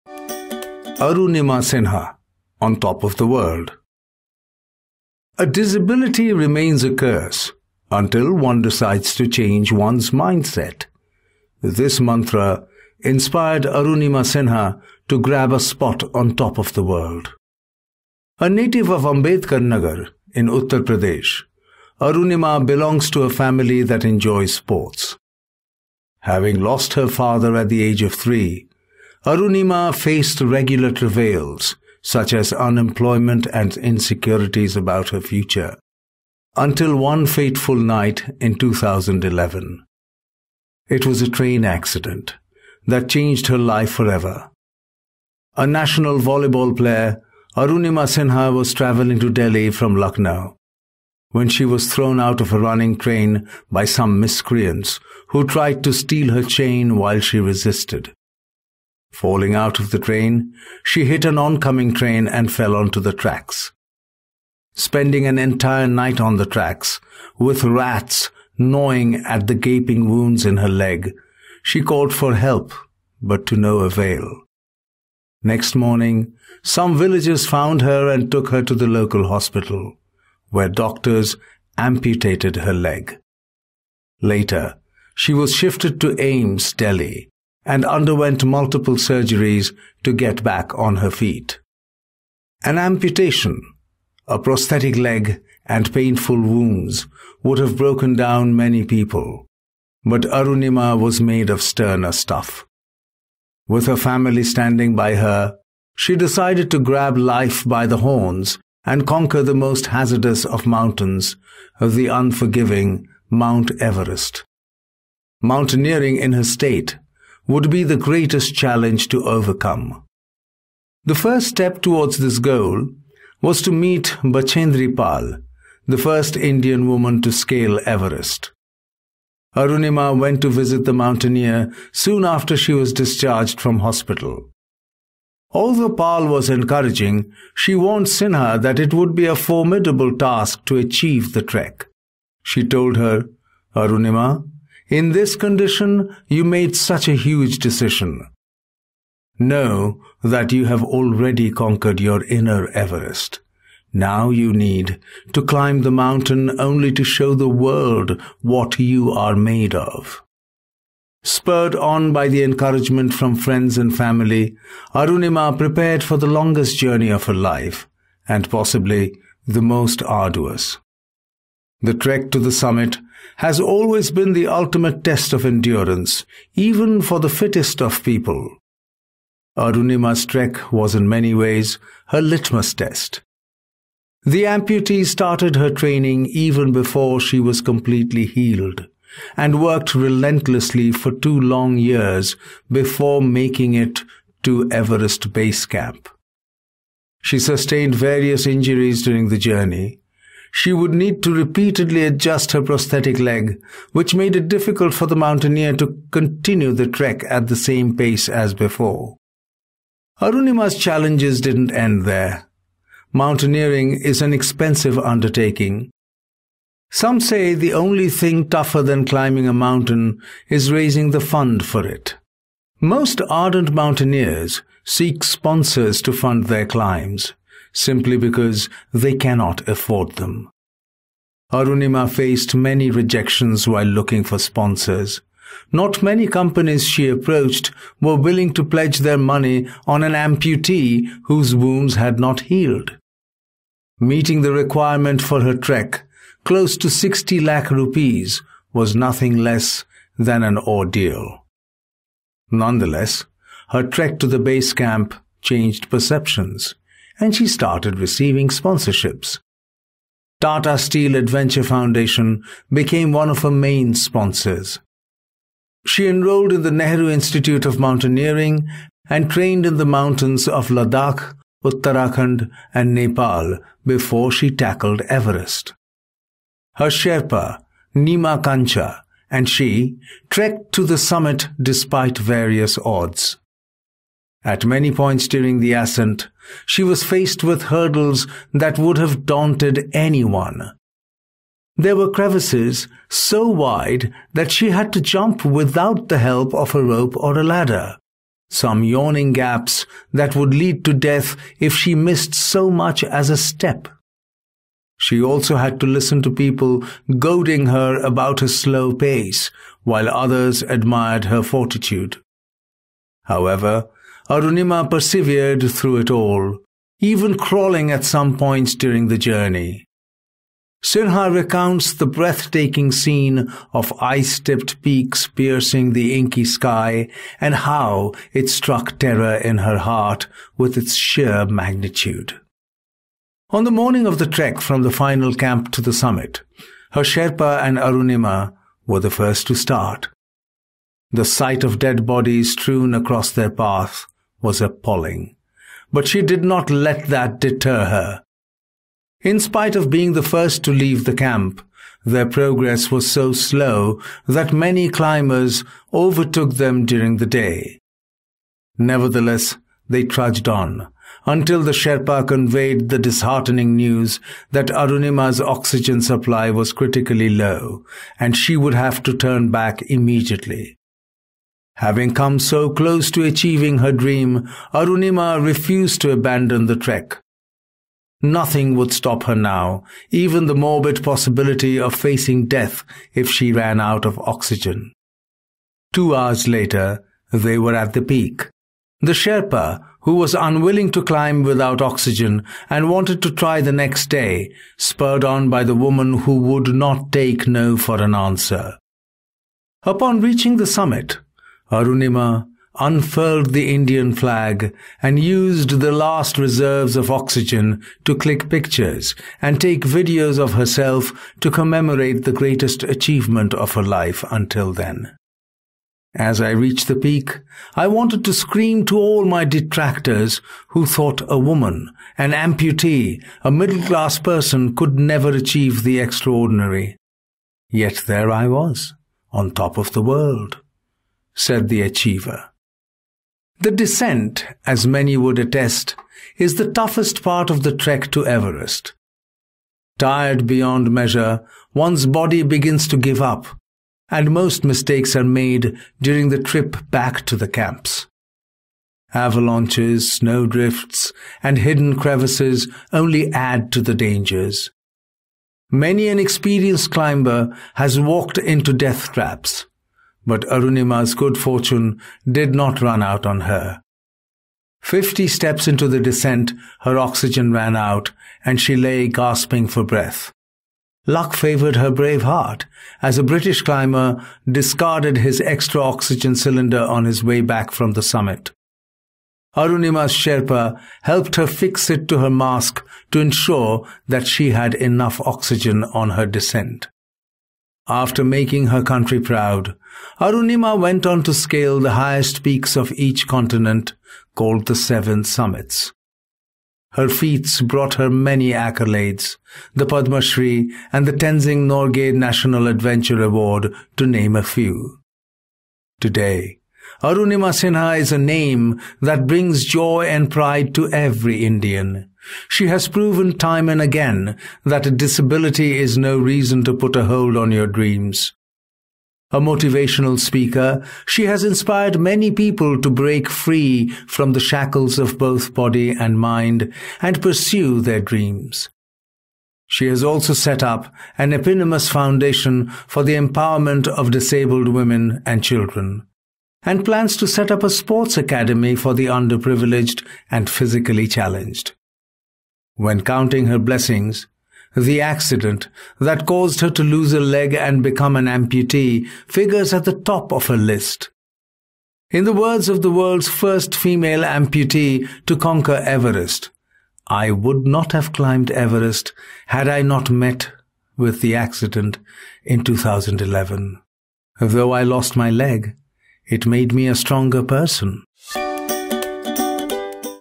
Arunima Sinha - Women's History Month
Let’s read another inspirational story of a young woman who braved challenges of all sorts, to conquer her dreams! Swipe to read more about her inspiring journey.